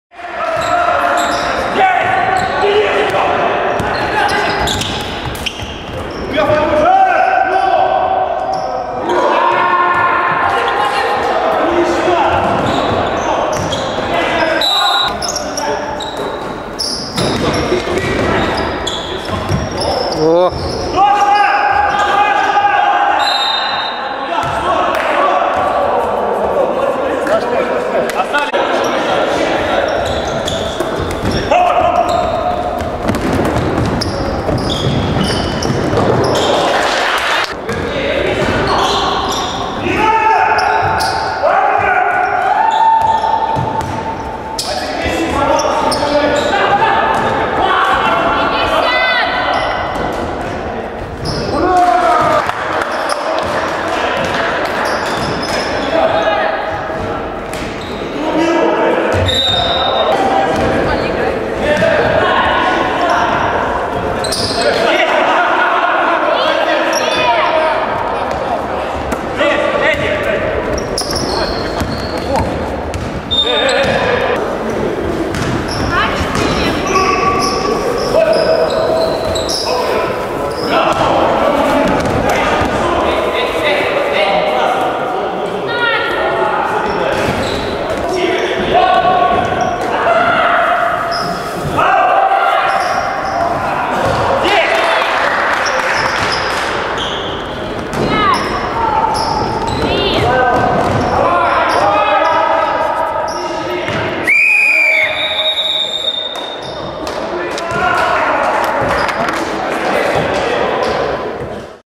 20.12.2005. КФК. Луч - Москва Баскетбольная.
Встреча между Лучом и МБ в рамках регулярного чемпионата КФК.